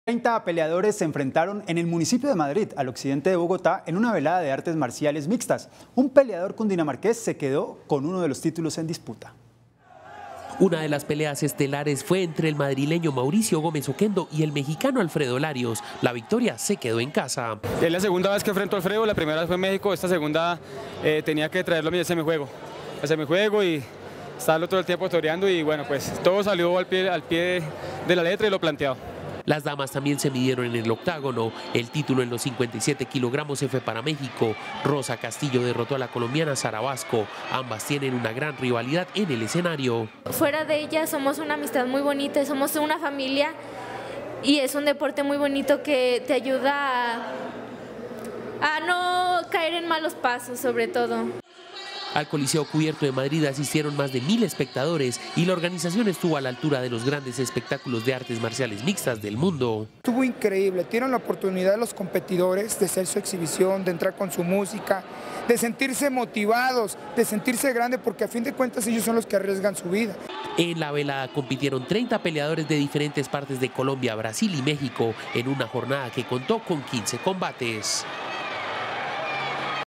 30 peleadores debutaron en la velada de artes marciales mixtas en Madrid, Cundinamarca
NoticiasCapital | Las peleas de artes marciales tuvieron como protagonistas a peleadores de distintas nacionalidades como México. El peleador colombiano Mauricio Gómez fue uno de los destacados de la jornada.